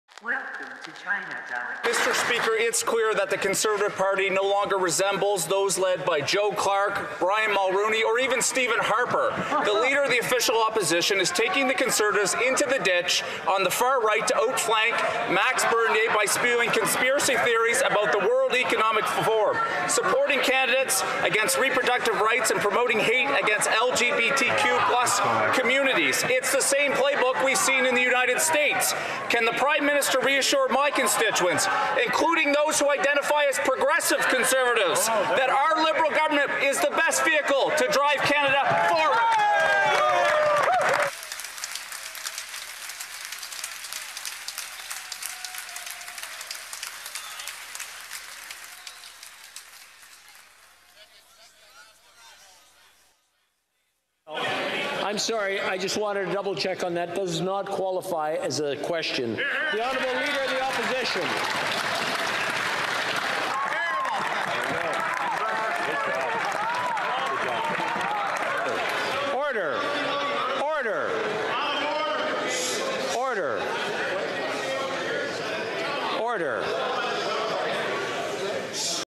Swamp Creature Shows Up In Parliament😱😱 (Swamp Edition)
Liberal Swamp Creature Shows Up In Parliament😱😱 (Swamp Edition)...

Kody Blois Loses His Mind, Starts Yelling About WEF & Attacking Poilievre🤡🤡...LIEberals Vitriolic Ideology On Display...

💎APPAREL💎

🌐 Pick up some drip from our clothing company. Premium streetwear apparel.

💎CRYPTO💎

💥BTC💥
3LvgCtDdh4wGGCfkfHgwB28rbpjkpibpLg

💥ETH💥
0xc794345ae72AB218dD3A8D0Cd619BB806Fe771cc

Chinada Official.